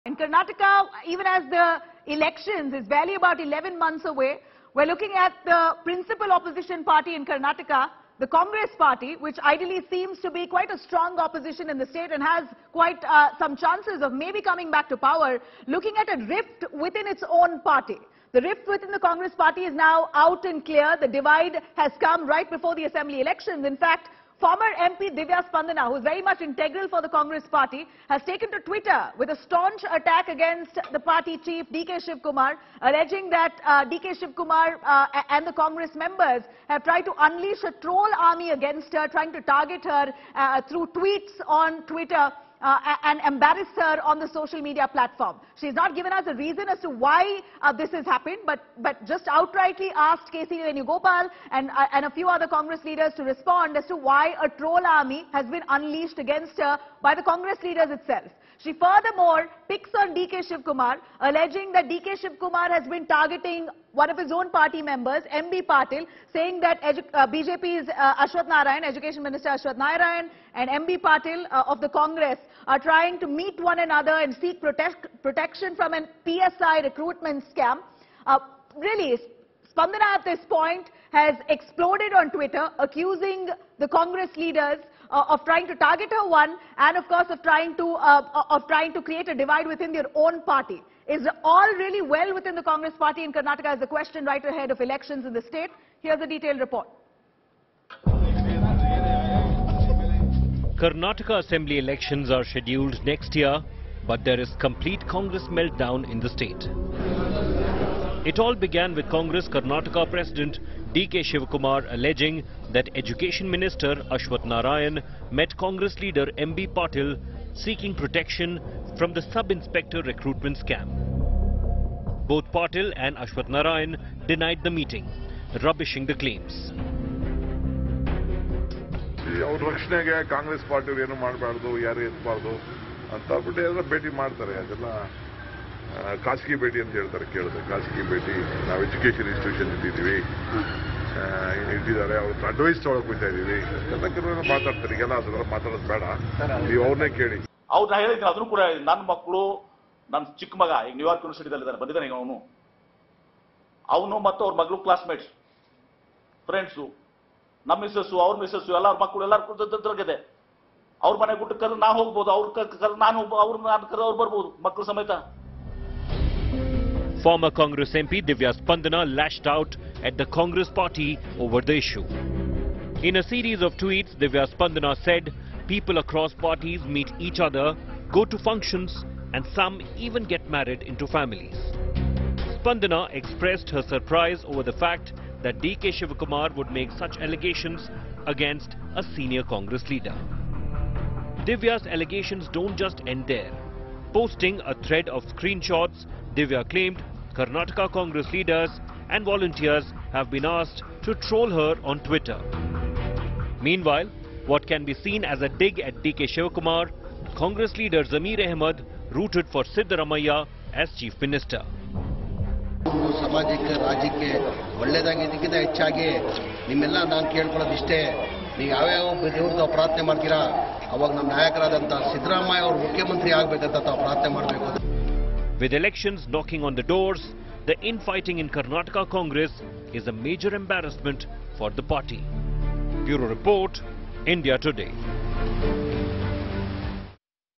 Karnataka Congress Rift Widens Just 11 Months Ahead Of Elections | Divya Spandana Vs DK Shivakumar
The rift in the Congress party is out in the public and it is widening day by day. Former Congress social media head and former MP Divya Spandana took to Twitter recently and said that members of Congress were being asked to ‘troll her’. Watch full bulletin.